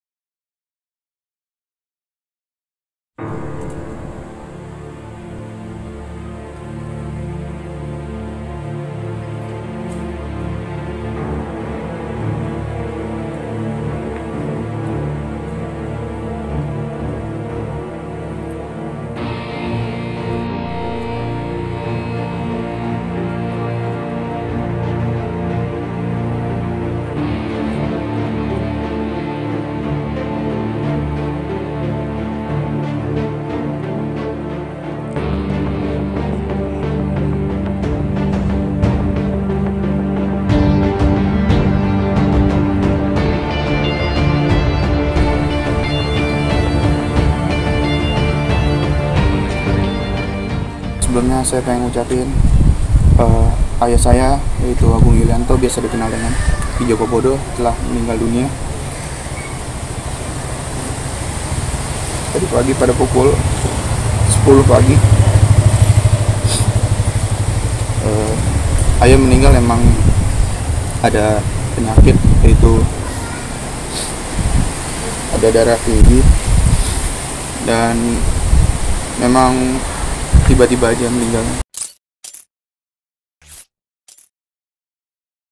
KI JOKO BODO MENINGGAL DUNIA, BEGINI SITUASI DI RUMAH DUKA
Dunia hiburan tanah air berduka, sosok Ki Joko Bodo yang dulu dikenal sebagai paranormal meninggal dunia. Hal tersebut dikonfirmasi oleh anak Ki Joko Bodo, Ayda Prasasti lewat sambungan telepon pada Selasa, (22/11) pukul 10.00 WIB.

Ki Joko Bodo meninggal pada usia 59 tahun, ia juga telah hijrah dan menjadi sosok religius.

Dalam kondisi menangis, Ayda meminta awak media untuk tak membuat berita buruk mengenai ayahnya.